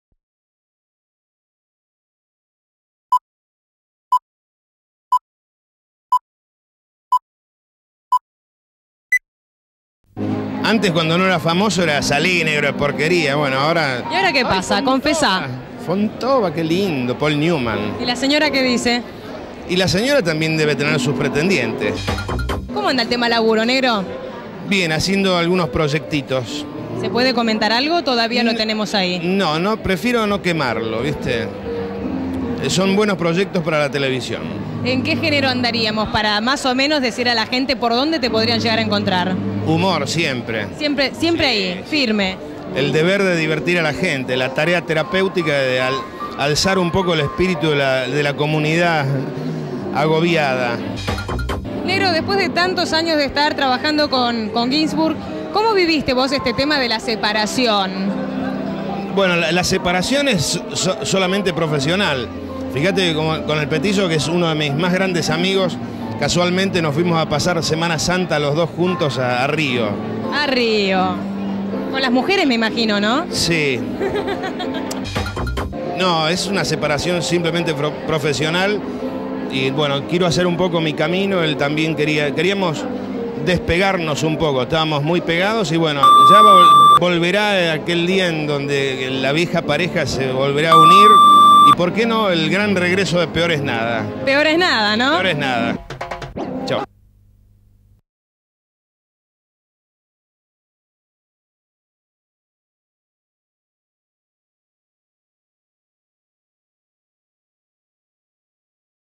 Horacio Fontova deja el ciclo televisivo Peor es Nada 1996
Bs. As.: Reportaje a Horacio Fontova, en el cual se le pregunta: -¿Cómo anda el tema de trabajo? -¿Negro, después de tantos años de estar trabajando con Jorge Guinzburg, como viviste vos este tema de la separación?
Fecha: 9/4/1996
Duración: 1 minuto 27 segundos
BG-0279